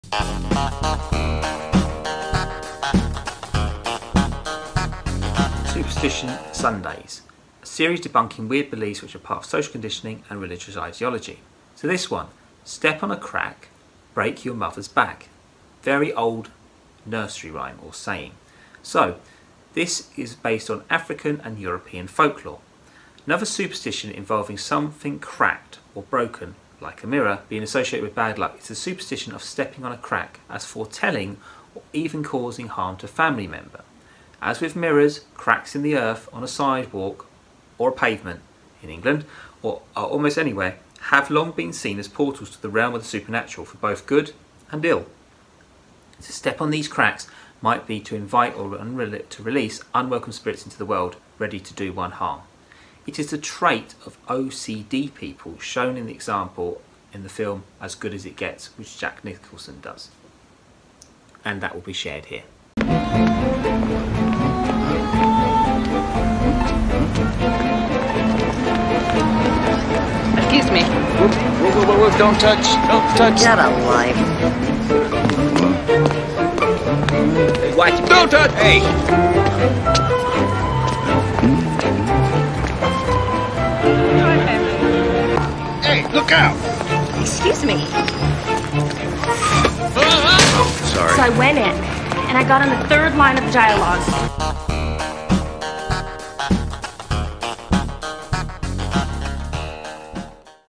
Superstitious Sunday: Debunking weird beliefs part of social conditioning: Step on a crack.........
sunday A series debunking weird beliefs which is part of conditioning.

“Step on a Crack, Break Your Mother’s Back”African and European folkloreAnother superstition involving something cracked or broken being associated with bad luck is the superstition of “stepping on a crack” as foretelling, or even causing, harm to a family member.

As with mirrors, cracks—in the earth, on a sidewalk, or almost anywhere—have long been seen as portals to the realm of the supernatural, for both good and ill.

To step on those cracks might be inviting or releasing unwelcome spirits into the world ready to do one harm.

Its a trait of ocd people shown in this example in the film